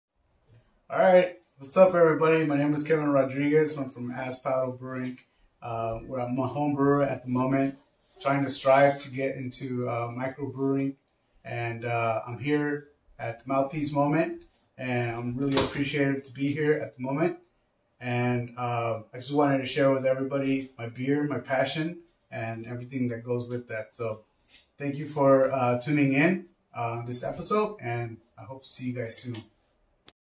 Mouthpiece Moment - Kevin Rodriguez Episode #79
THIS IS A BEER TASTING EPISODE!

On this episode, Kevin and Anthony return to the show and Jose makes his debut. Kevin owns and operates Asspaddle Home Brewers and was generous enough to bring us a bunch of beer! Kevin brought 2 of his new beers called "Wit-tier" and another unnamed Peach Brut IPA. We gave them sevvvvvvveral tries and also took a shot for Kevin's birthday. Kevin gave us a little history lesson on Witbier and even brought ingredients he used to create these beers, for us to try before tasting the beer. Jose tells us some crazy stories about living in South Central during the riots last year and we all make several connections amongst each other. Anthony finished his apprenticeship and is now officially, a Journeyman Lineman. I had several questions and topics to bring up, be we were having so much fun that I didn't use my notes much.

Check Out this DOPE Clothing Brand - Into the AM

Tommy Chong's CBD!
Save 20% with code TOMMY20!